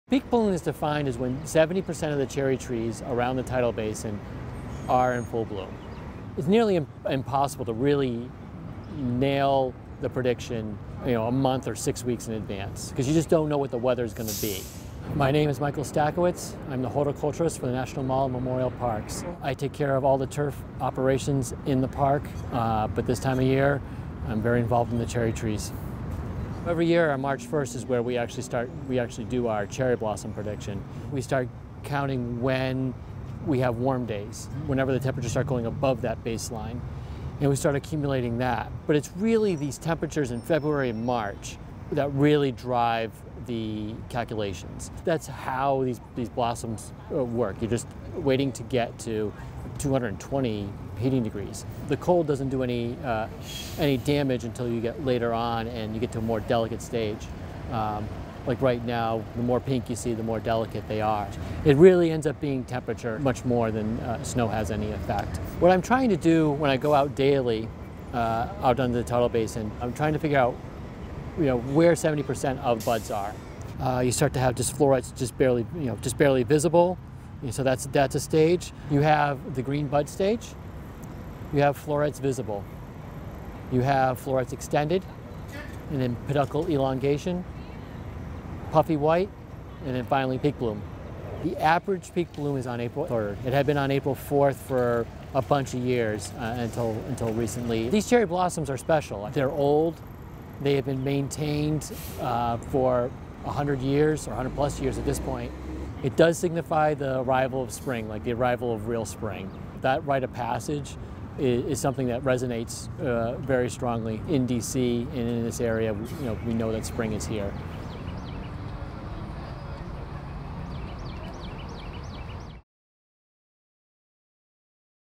When will D.C.'s cherry blossoms reach peak bloom? It's this man's job to know.
Michael Stachowicz of the National Park Service evaluates weather data and looks at cherry trees on the Tidal Basin to come up with peak bloom dates.